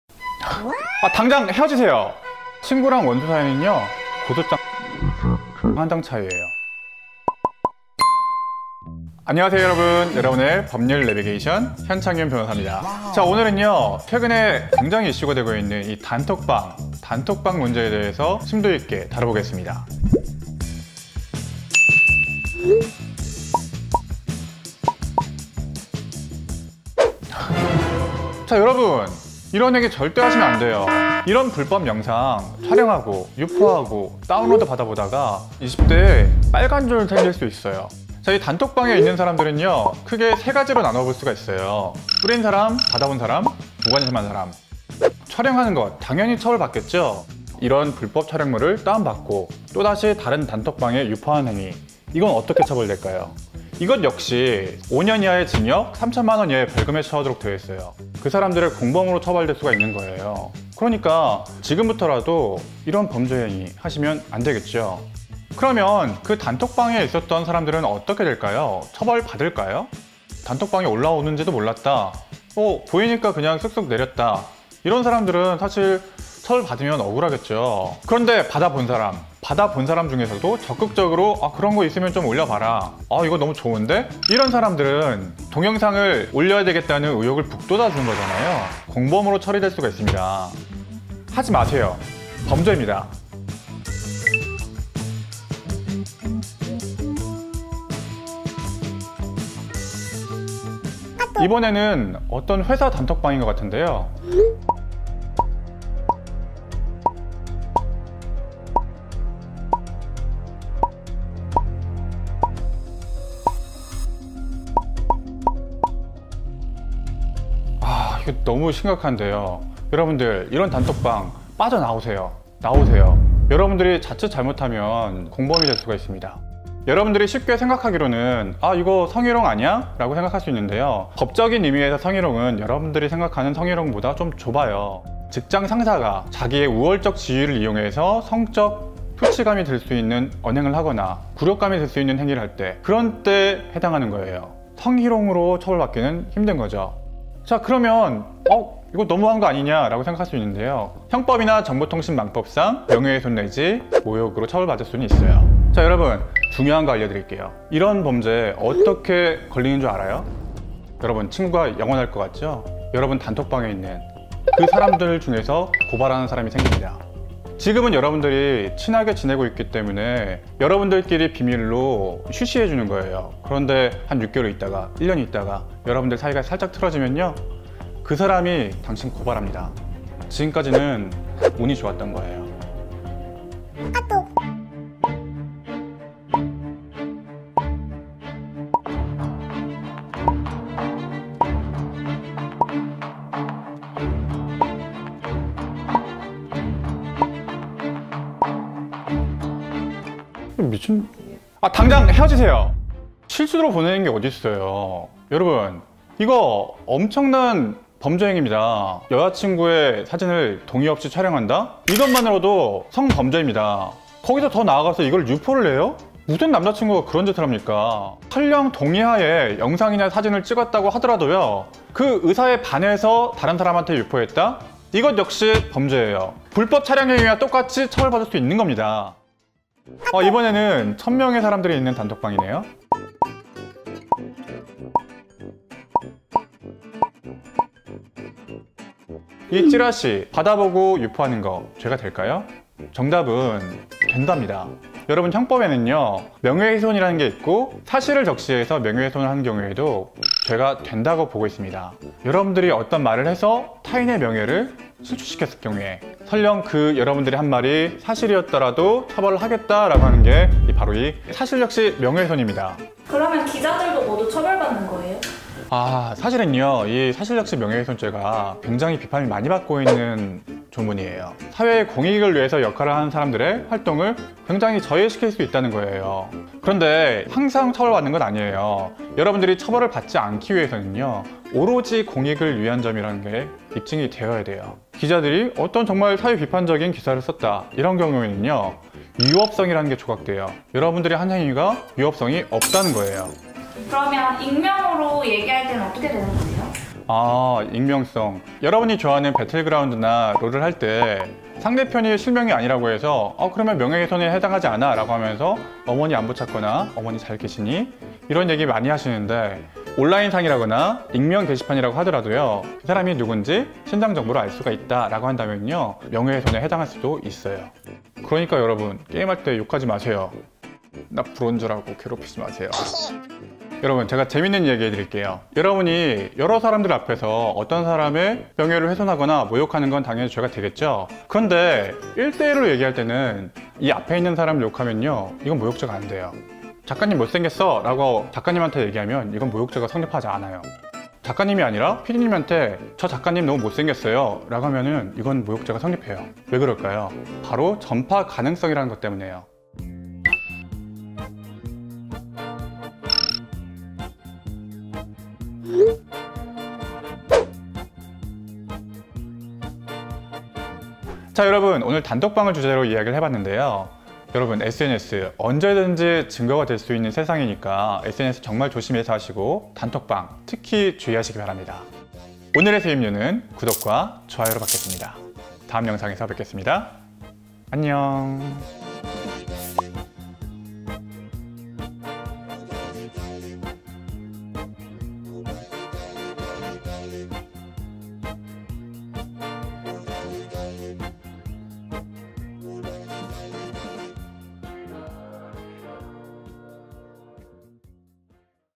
단톡방에서 하면 안되는 ㅇㅇㅇ... 이걸 했다면.. 고소각?!?! / 14F
"동영상 신작 떳다!"
"헐! 얼른 공유해!"

단톡방에서 일어나는 모든 범죄!
지금까지는 운 좋아서 안 걸린 거예요...